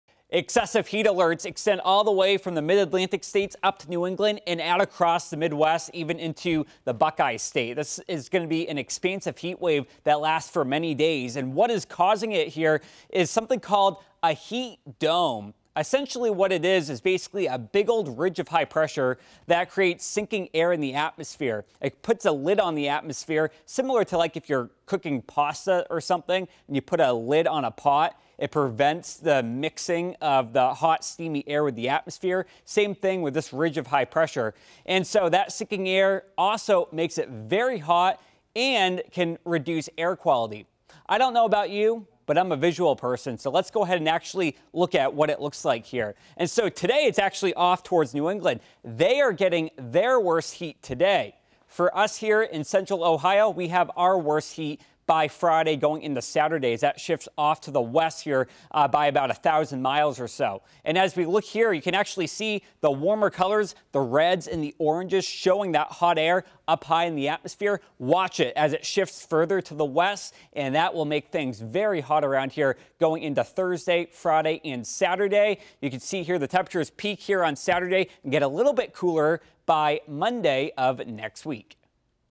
What are heat domes and what causes them?
Doppler 10 Meteorologist Dylan Robichaud explains what a heat dome is and how it affects the temperatures in Ohio.